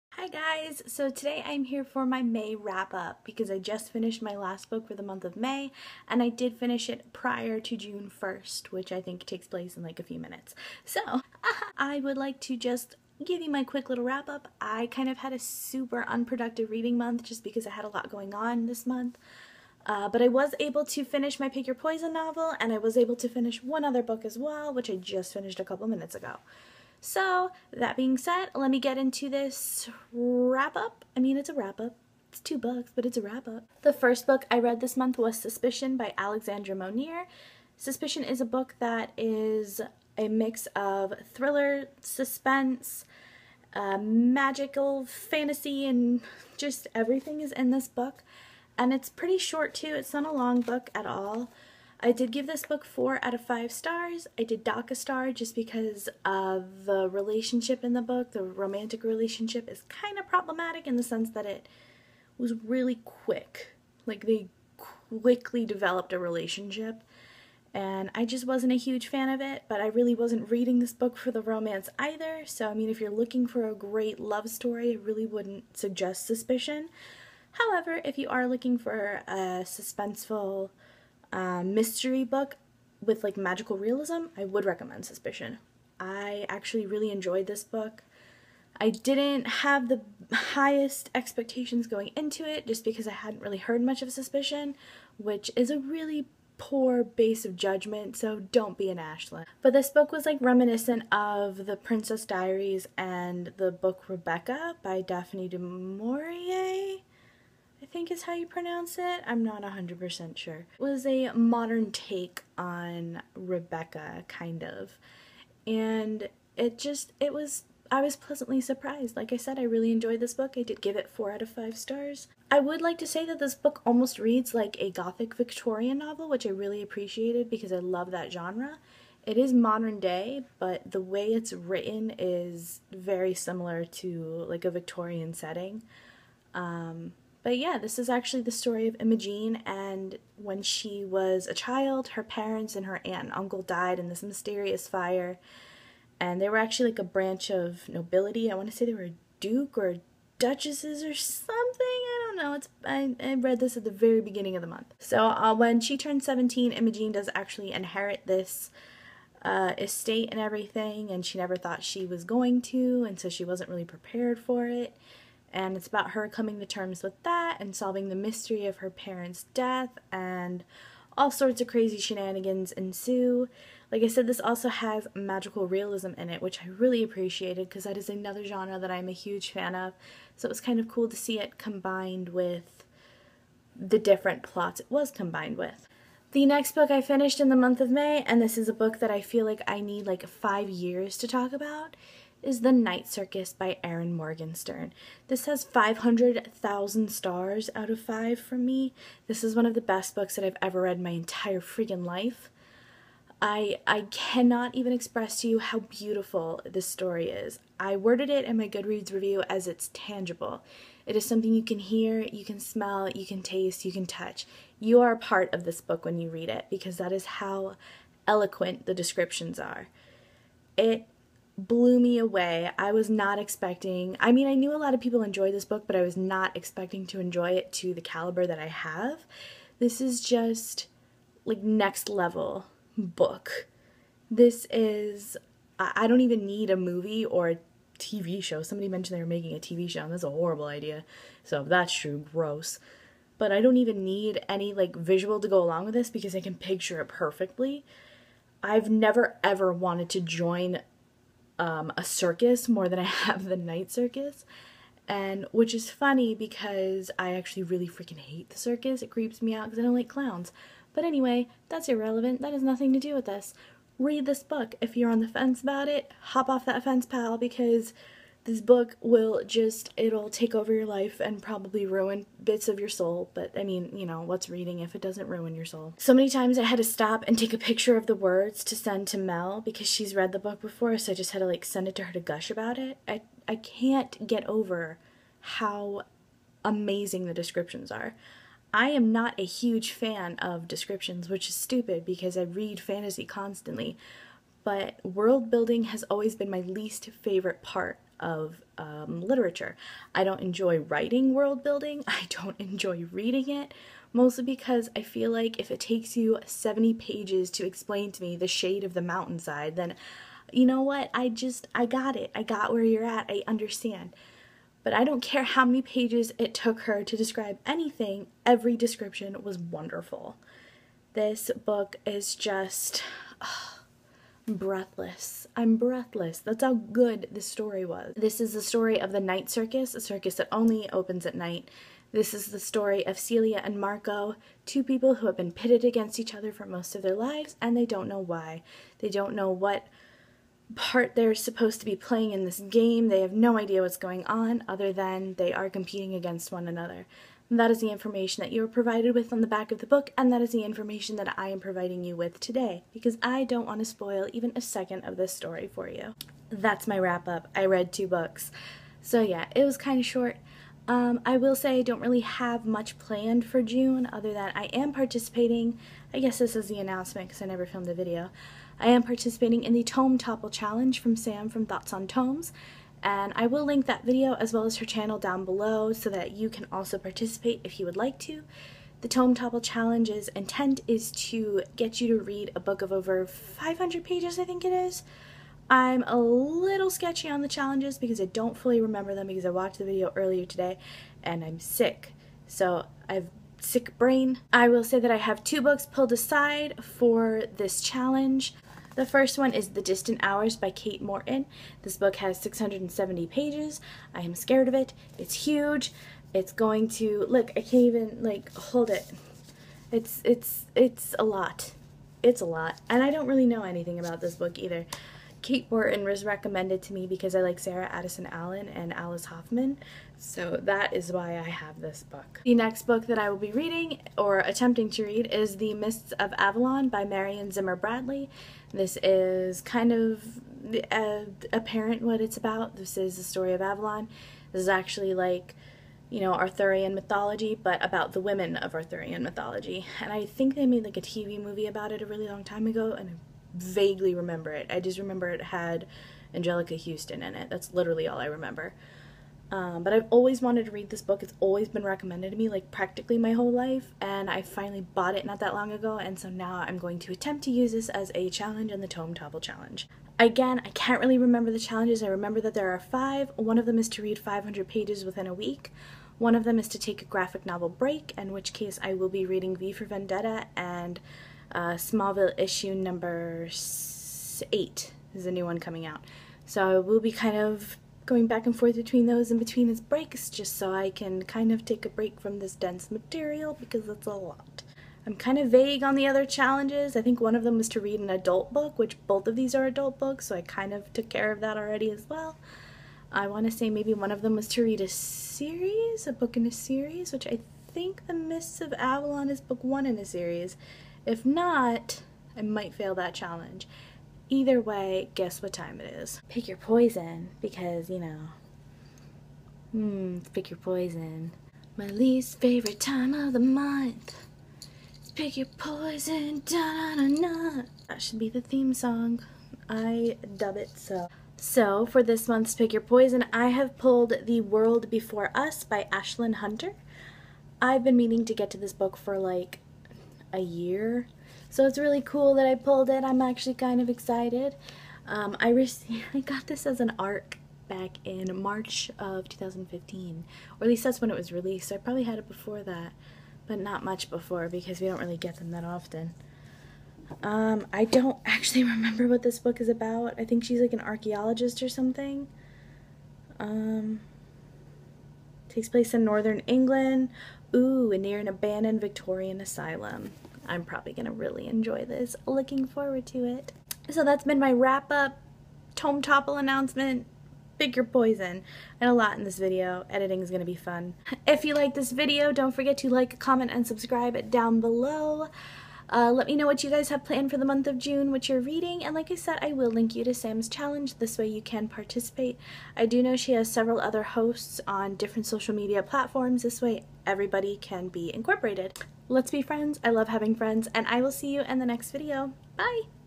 ALL THE THINGS: Wrap Up/PYP/TomeTopple
IM SICK. AGAIN.

Books Featured:
Suspicion by Alexandra Monir
The Night Cirus by Erin Morgenstern
The Distant Hours by Kate Morton
The Mists of Avalon by Marion Zimmer Bradley
The World Before Us by Aislinn Hunter